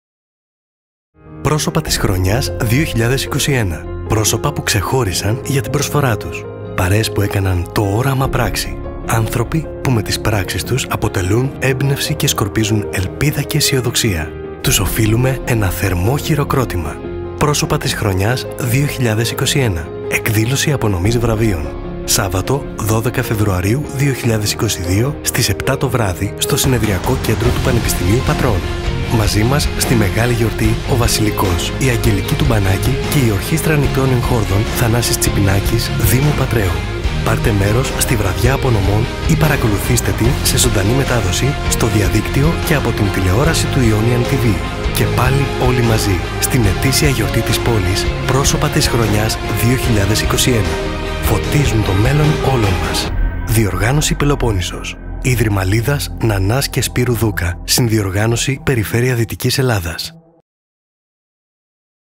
ΠΡΟΣΩΠΑ ΧΡΟΝΙΑΣ 2021 – Trailer Εκδήλωσης απονομής βραβείων
H εφημερίδα ΠΕΛΟΠΟΝΝΗΣΟΣ διοργανώνει για 8η συνεχόμενη χρονιά τα «Πρόσωπα της Χρονιάς». Πρόκειται για μια θεσμοθετημένη πλέον εκδήλωση στην αχαϊκή κοινωνία, που πραγματοποιείται στις αρχές του χρόνου και κατά την οποία τιμούμε συμπολίτες μας που επέδειξαν αξιόλογη δράση το έτος που πέρασε. Στόχος μας είναι η ανάδειξη και βράβευση ανθρώπων που αποτελούν παράδειγμα προς μίμηση, ο καθένας στον τομέα του, για τη σκληρή προσπάθειά τους, τα επιτεύγματά τους και την επιμονή να κάνουν το όραμά τους πράξη.